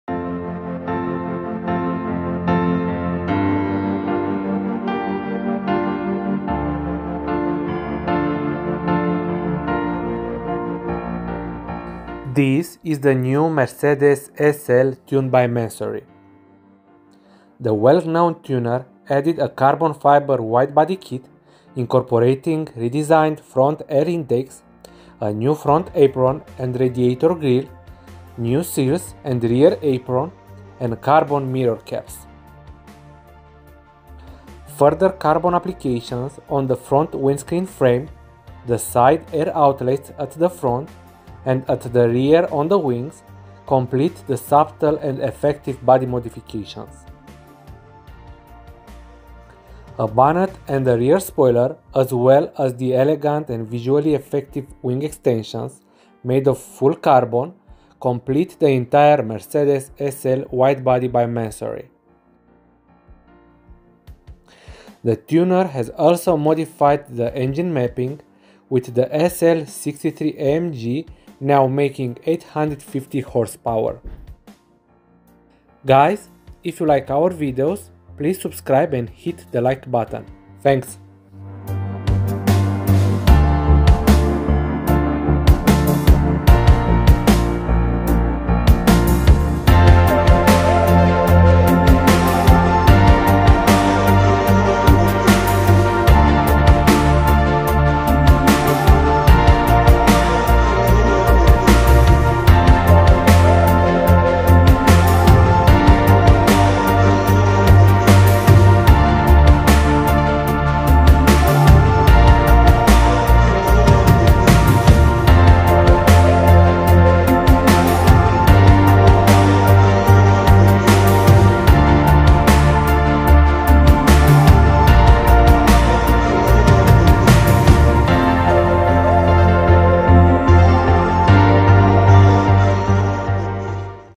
2024 MERCEDES SL tuned by MANSORY is a masterpiece!
This is the new Mercedes SL tuned by Mansory.

The well-known tuner added a carbon fibre wide-bodykit, incorporating redesigned front air intakes, a new front apron and radiator grille, new sills and rear apron, and carbon mirror caps.

Further carbon applications on the front windscreen frame, the side air outlets at the front, and at the rear on the wings, complete the subtle and effective body modifications.

A bonnet and a rear spoiler, as well as the elegant and visually effective wing extensions made of full carbon, complete the entire "Mercedes SL Widebody by MANSORY".

The tuner has also modified the engine mapping, with the SL 63 AMG now making 850hp.